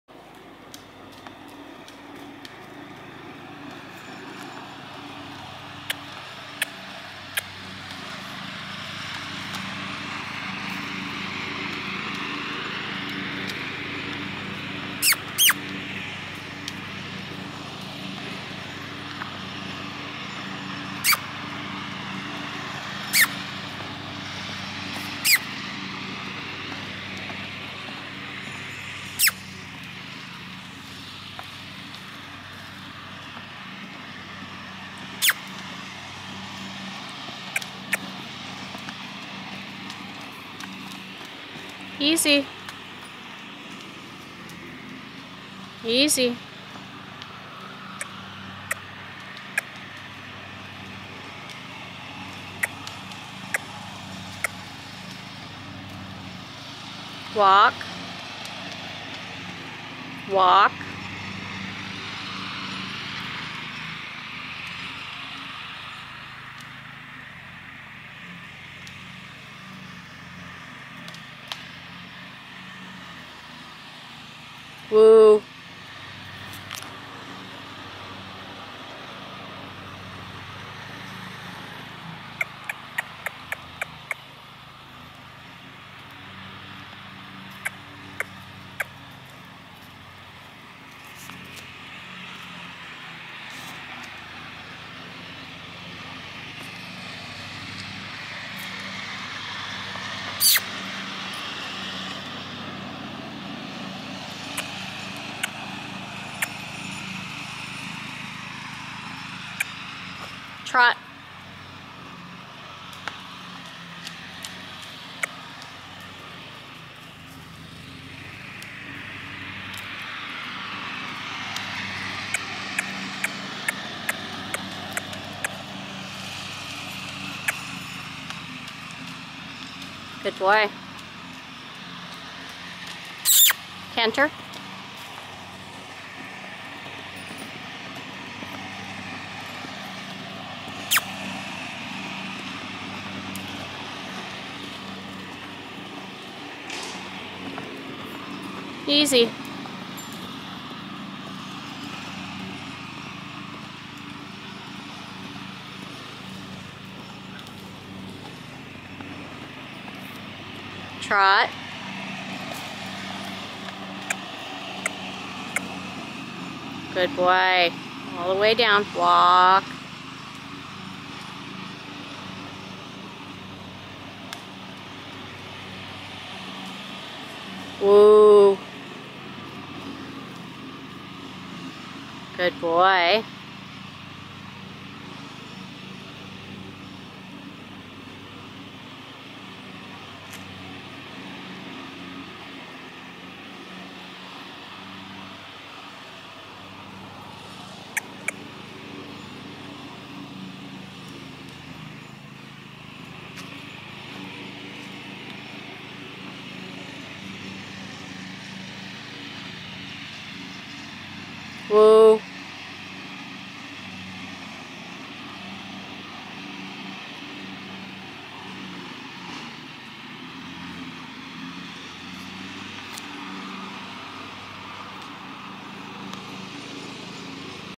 Certain To Sunspot
Certain To Sunspot.  2011 colt by Painted N Sunspots.
This colt is Homozygous for the Tobiano and Black genes.  He has also been tested 5 panel NN. He is registered with APHA and enrolled in the Breeders Trust program.  He was shown in long line as a yearling and turned out as a two year old to grow.  He has been started undersaddle as a 3 year old.